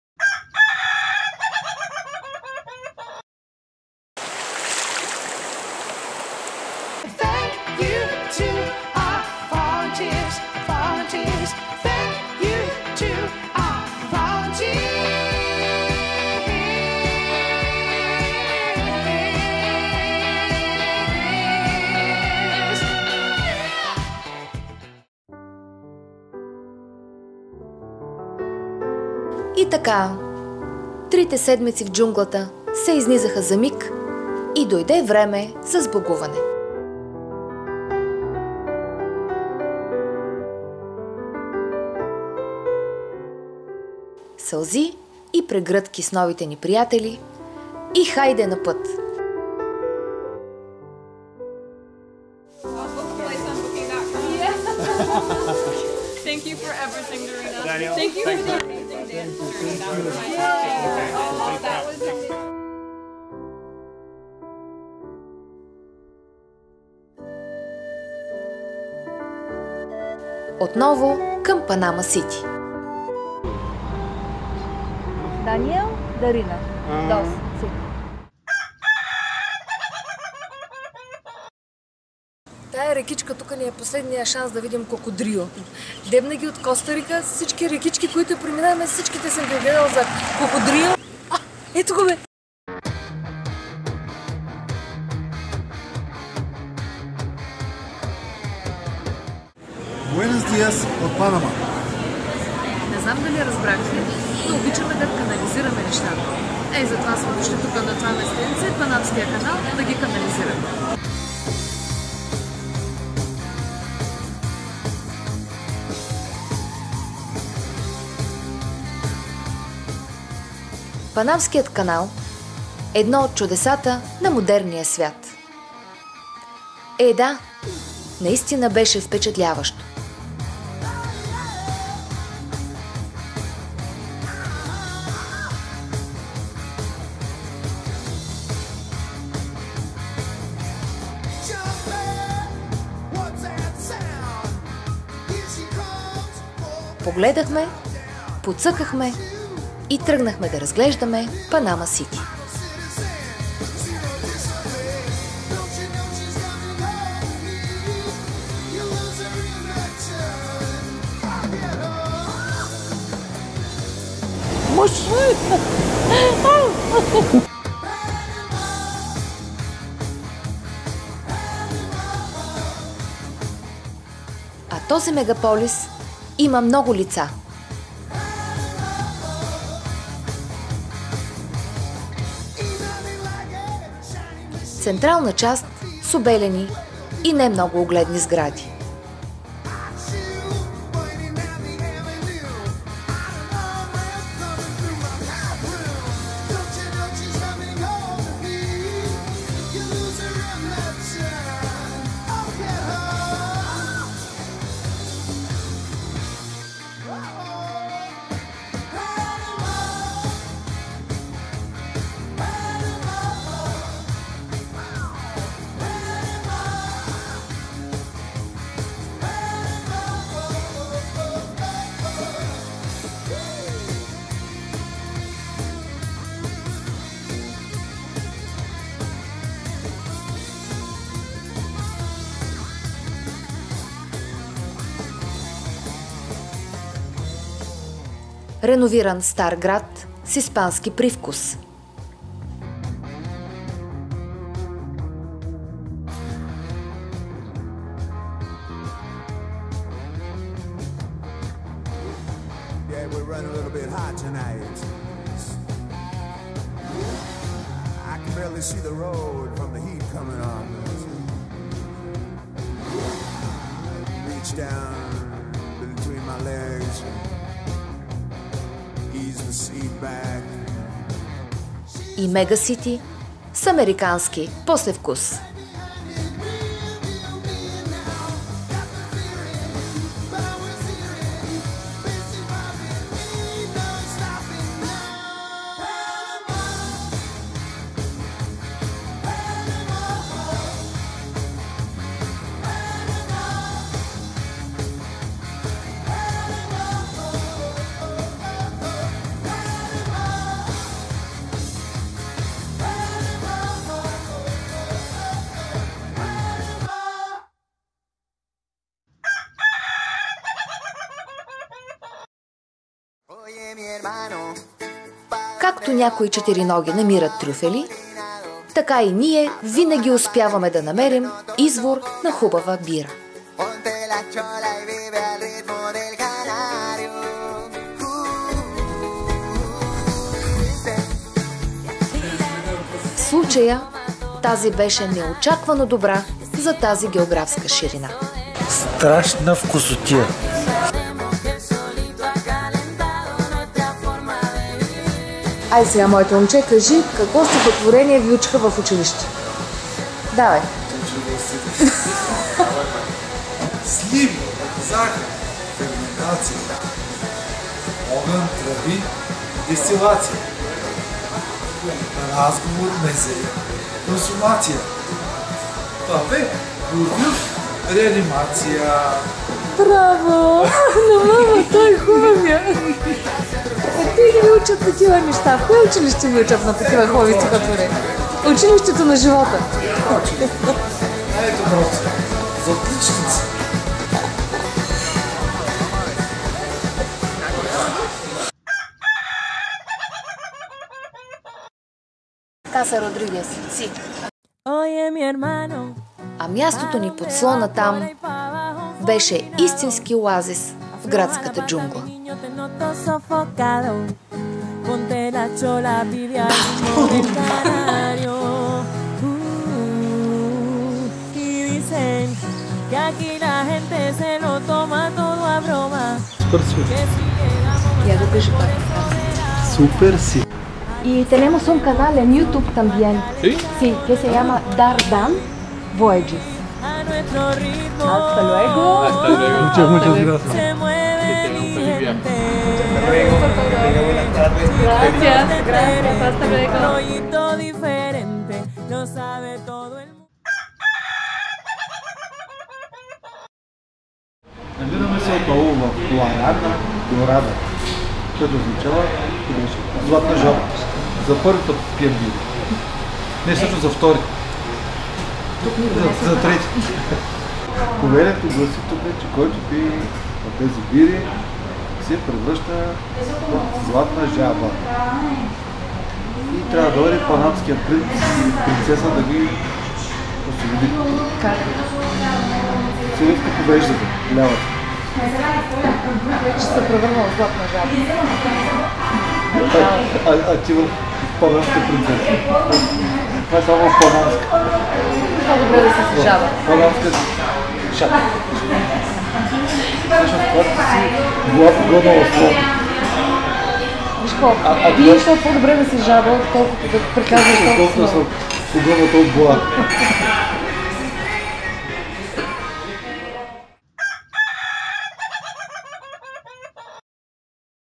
Episode 6 Panama canal, crocodile and golden frog w/ ENG sbtitles
Volunteer experience of Dardan in the jungle of Costa Rica, Finca Bellavista is over and they headed for Panama city - the last stop before they return home.